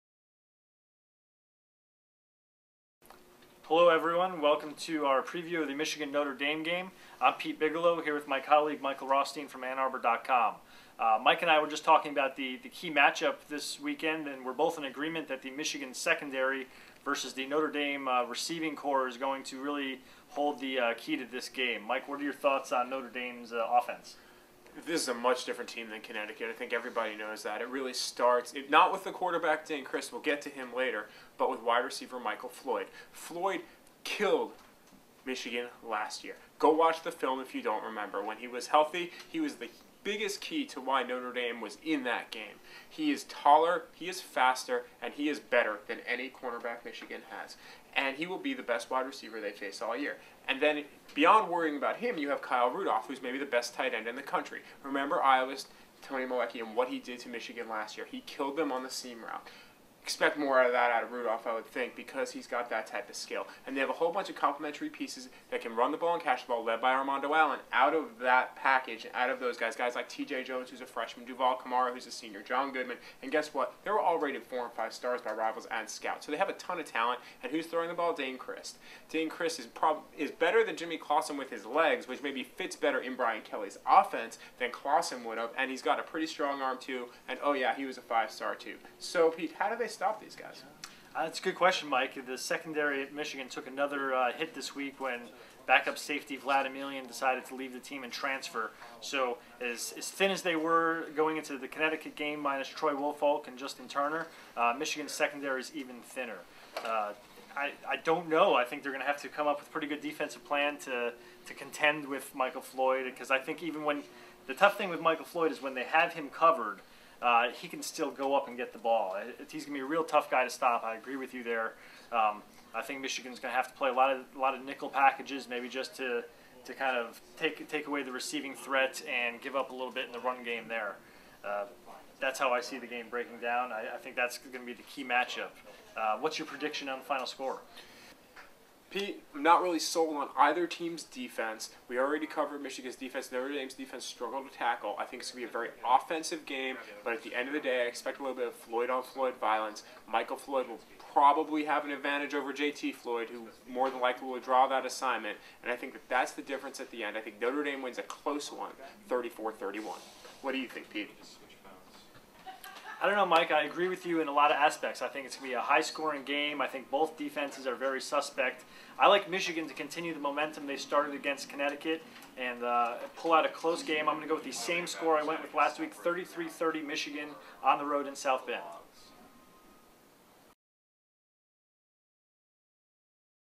Week 2: Michigan vs. Notre Dame preview
AnnArbor.com sports reporters Pete Bigelow and Mike Rothstein discuss the keys to Saturday's game between Michigan and Notre Dame.